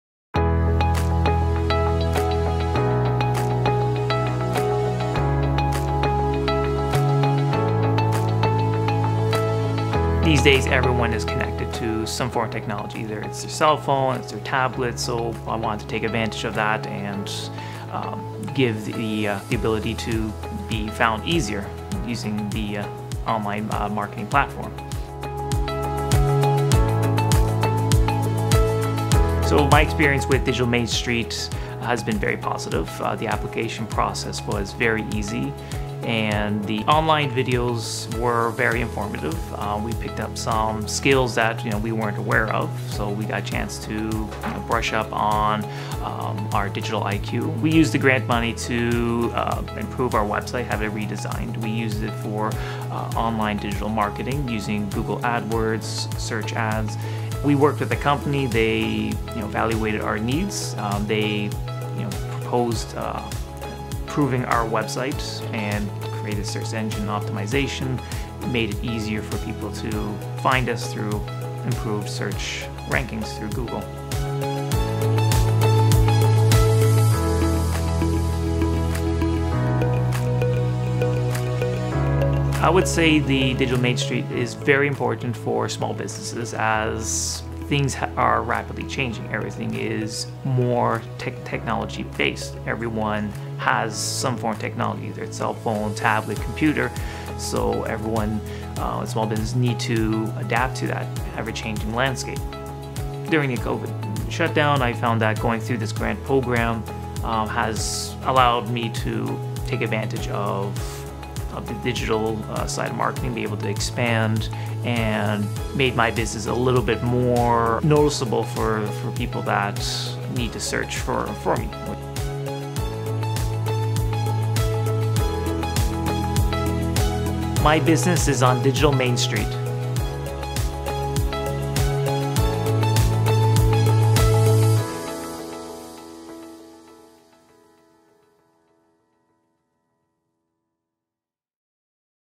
Digital Main Street - Digital Transformation Grant Testimonial - Dundas Denture Clinic, Dundas, ON
Digital Transformation Grant (DTG) Recipient – Emilio Leuzzi - Owner/Denturist, Dundas Denture Clinic, Dundas, ON.

The Dundas Denture Clinic has been fitting patients with custom dentures for more than 20 years, maintaining high quality at affordable prices. Learn how Emilio used the DTG funds to adapt to the ever-changing technology landscape by redesigning his website and investing in digital marketing. With his website now optimized for search and an increase in search rankings, more people i.e., potential customers, are finding Dundas Denture Clinic and the services they offer.

Dundas Denture Clinic is on Digital Main Street.